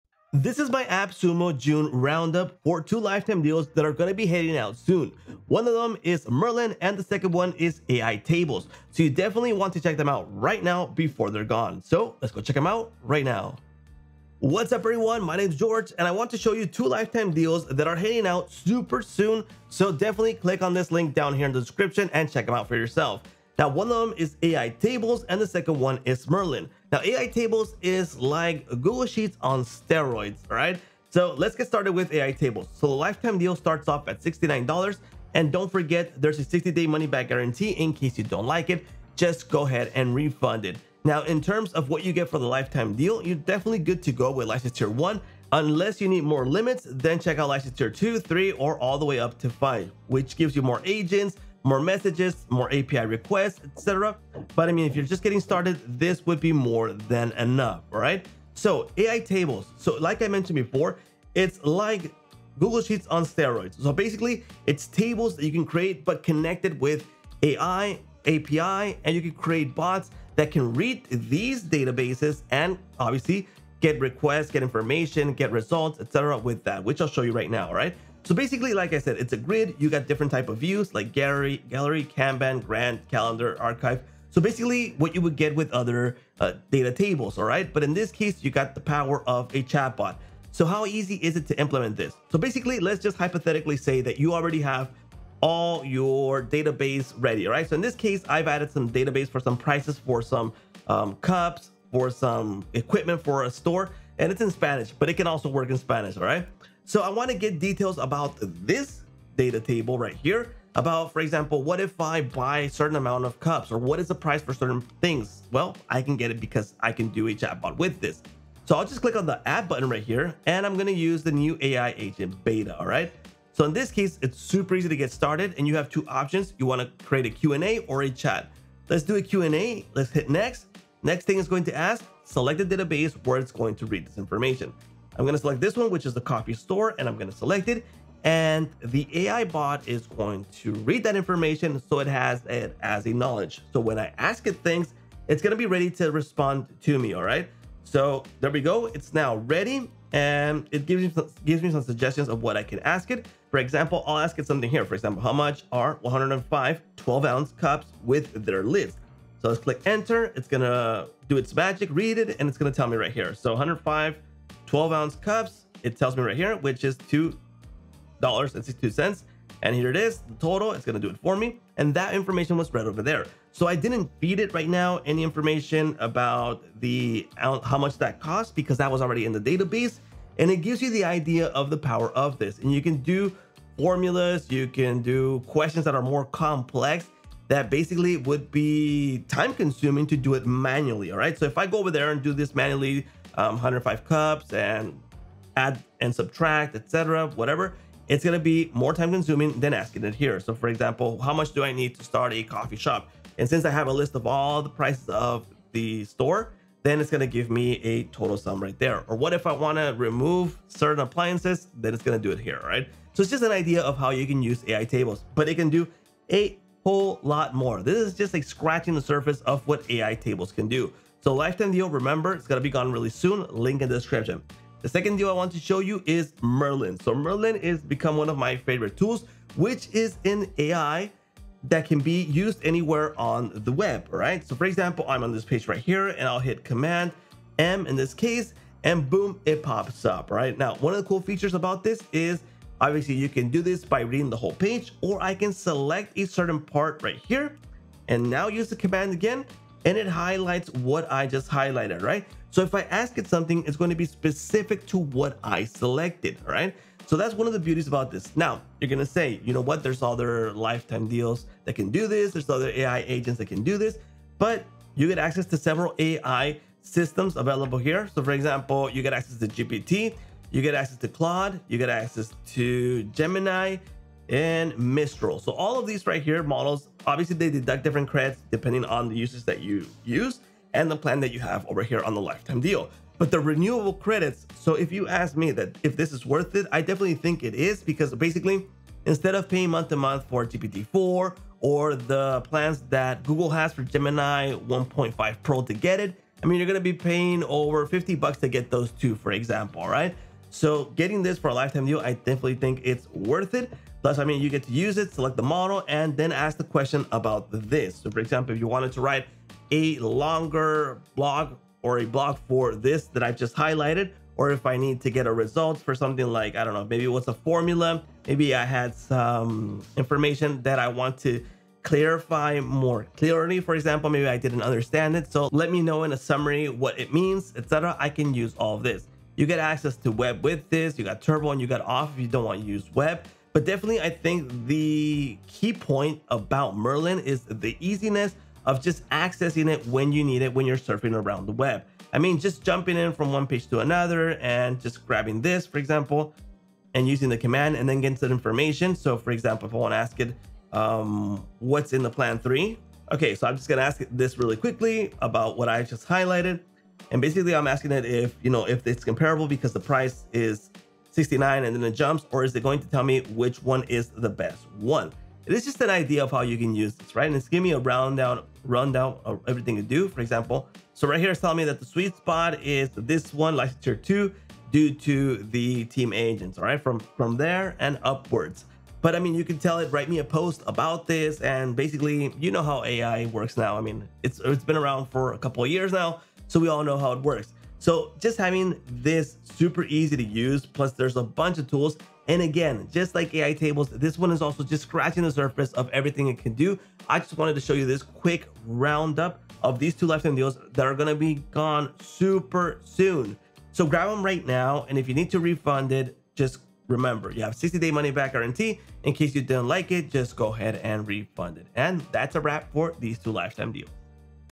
Don't Miss Out: AI Tables & Merlin AI Lifetime Deals! 🔥
Welcome to my AppSumo June Roundup, where I'll demonstrate two SaaS gems you can't afford to miss: **AI Tables** and **Merlin AI**. These lifetime deals are vanishing quickly, so grab them while you can!

**AI Tables** is like Google Sheets on steroids, offering robust AI-powered data management. Whether you're handling e-commerce data or SEO optimization, AI Tables boosts productivity with features like Q&A bots and complex formula calculations. It's your go-to tool for seamless digital signage and AI-generated imagery management.

**Merlin AI** is your web companion, integrating multiple AI systems like GPT-4, Claude, and Gemini for versatile online assistance. From writing blog posts to summarizing complex data, Merlin AI makes web navigation and information processing a breeze.

🌟 **Why You Should Act Fast:**
- **Lifetime Access:** Say goodbye to recurring fees.
- **60-Day Money-Back Guarantee:** Risk-free exploration.
- **Exclusive Features:** Unlock advanced capabilities in e-commerce and data management.

Don't wait! Discover the power of AI Tables and Merlin AI today. 🚀

CHAPTERS:
00:00 - Intro
00:32 - AI Tables Lifetime Deal
03:44 - Merlin AI Overview
06:37 - Final Thoughts